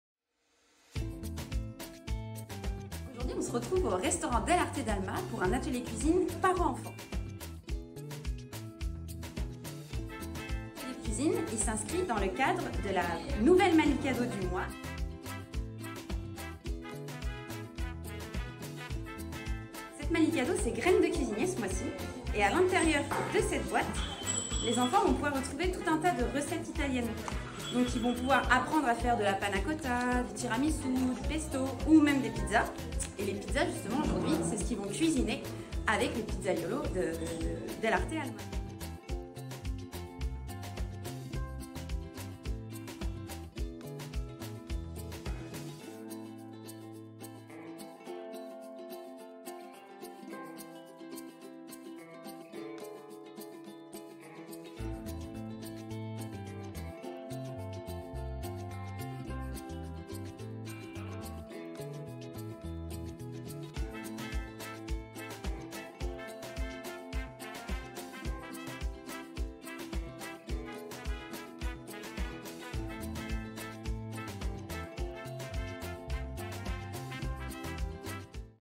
L'atelier cuisine Del Arte, entre parents et enfants - Groupe LE DUFF
🍕👨‍🍳 Les petits chefs en herbe se sont essayés à l’art de la pizza lors de notre atelier cuisine Parents / Enfants dans notre @delartefr Rennes Alma !

Un moment délicieux permettant de découvrir la Malicado Graine de Cuisinier disponible dans nos restaurants Del Arte avec de belles recettes italiennes à reproduire en famille à la maison.

👏 Un grand bravo à nos jeunes pizzaiolos et à leurs parents pour leur enthousiasme et leur bonne humeur.